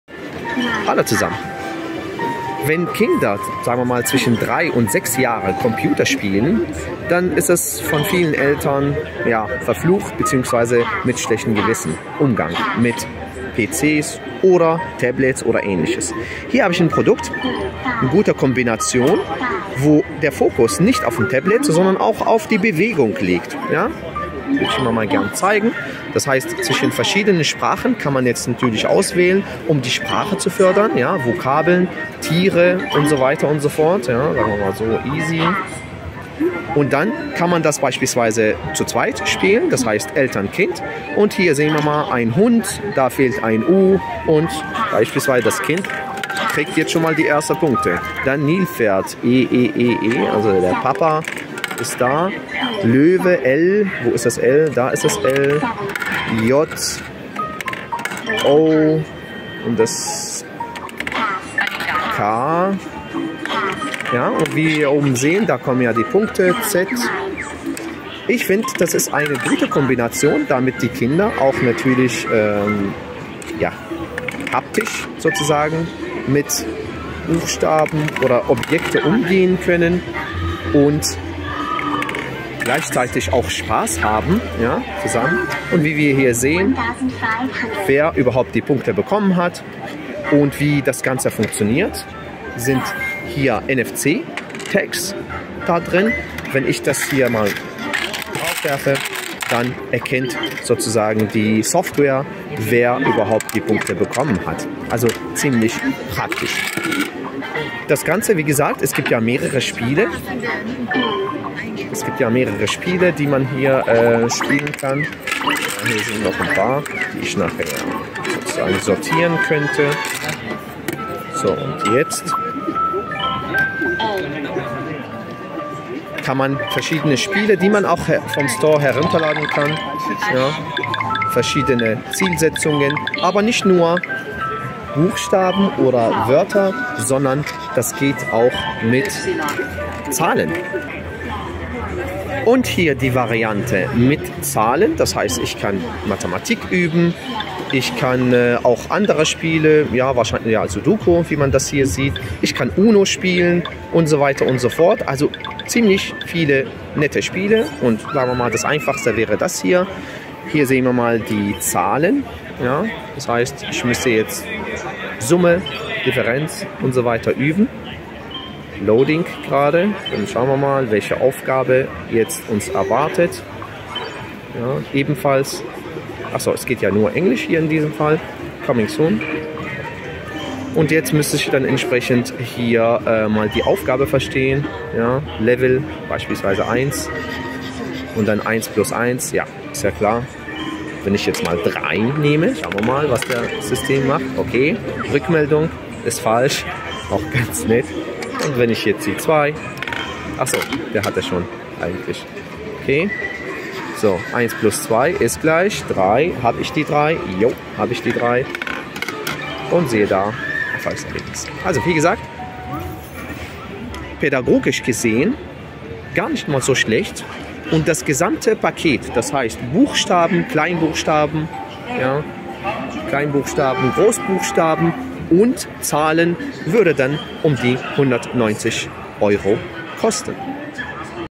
Kindergerechte Computer-Spiele
in diesem Video werden pädagogisch-durchdachte Computerspiele gezeigt, wo Eltern mit ruhigem Gewissen zusammen mit ihren Kindern spielen können. Dabei gibt es verschiedene Zielsetzungen (Sprache, Rechnen, Logik usw.), wo die Kinder spielerisch lernen können. Das Ganze ist kombiniert mit Bewegung, damit auch die Motorik unterstützt wird.

00:00 - Allgemein und Sprache/Wortschatz üben
02:21 - Rechnen üben